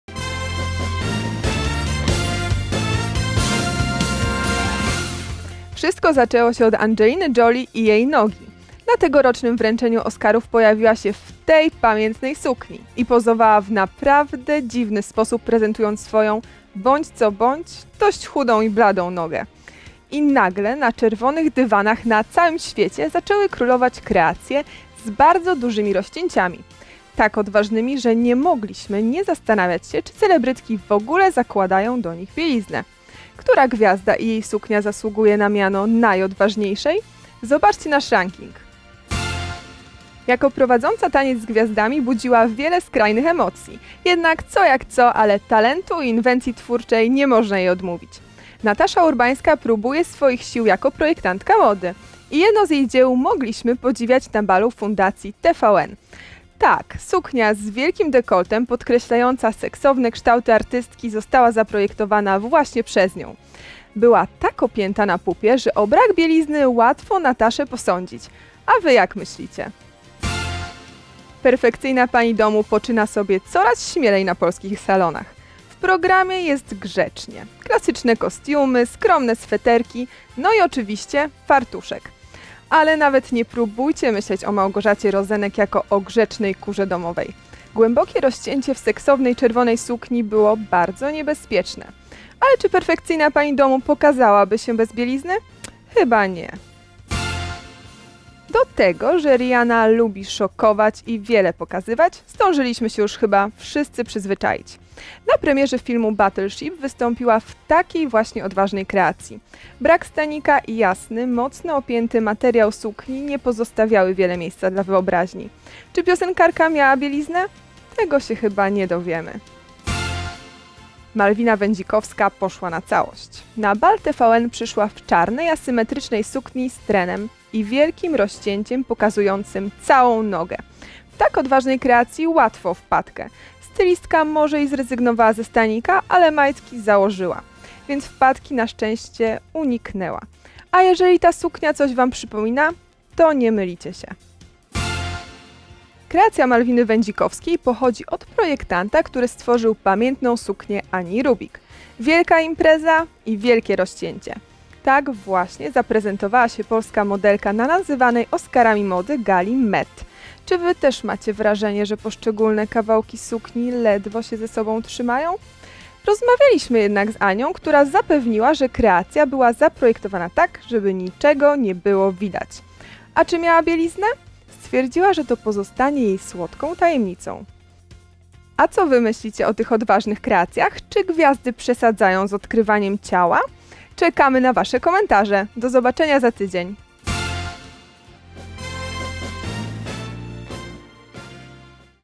Czy one założyły majtki?
plotek.pl: Wszystko zaczęło się od Angeliny Jolie i jej nogi. Na tegorocznym wręczeniu Oscarów pojawiła się w tej pamiętnej sukni i pozowała w naprawdę dziwny sposób prezentując swoją, bądź co bądź, dość chudą i bladą nogę. I nagle na czerwonych dywanach na całym świecie zaczęły królować kreacje z bardzo dużymi rozcięciami. Tak odważnymi, że nie mogliśmy nie zastanawiać się, czy celebrytki w ogóle zakładają do nich bieliznę. Która gwiazda i jej suknia zasługuje na miano najodważniejszej? Zobaczcie nasz ranking! Więcej wideo